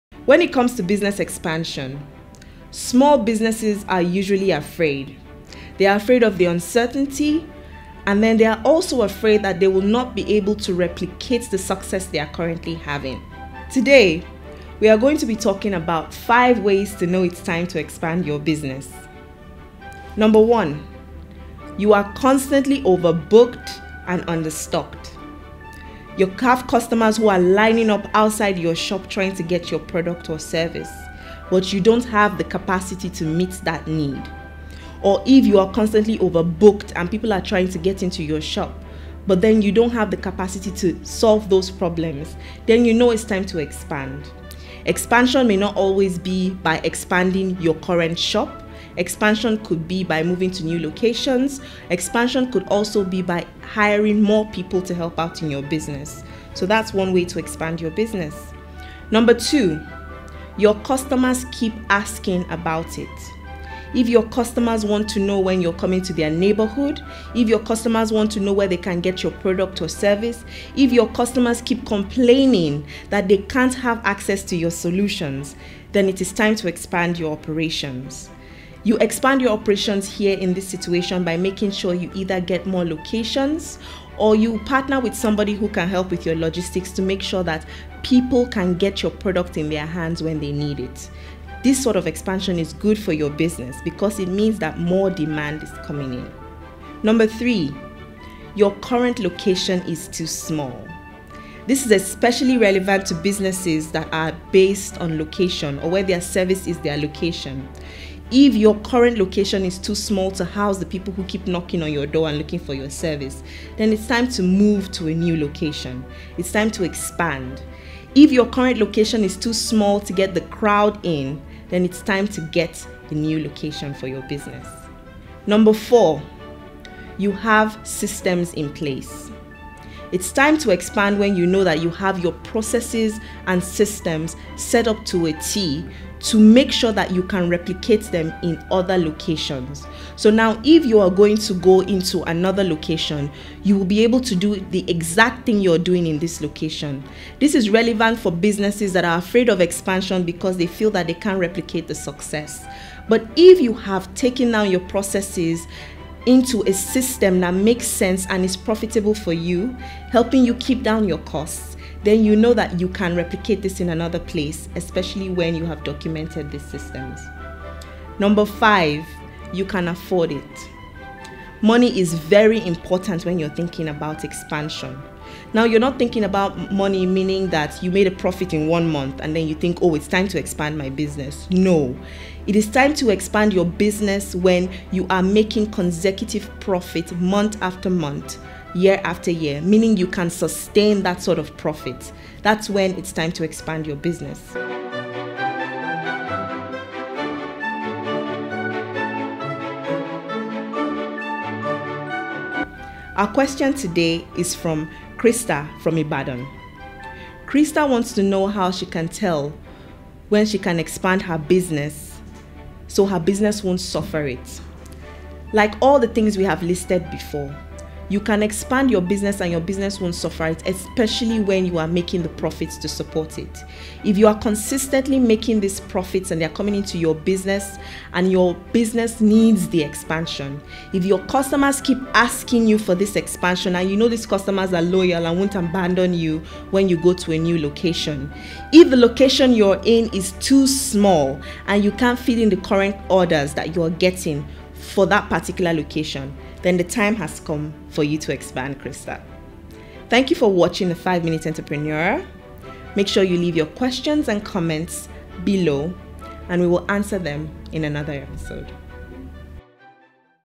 How to know when it's time to expand your business | 5 Minute Entrepreneur
Small businesses are usually unaware of when it's time to expand. In this episode of 5 Minute Entrepreneur, Business Strategist, Koyona Duke tells us five ways to know when it is time to expand your business.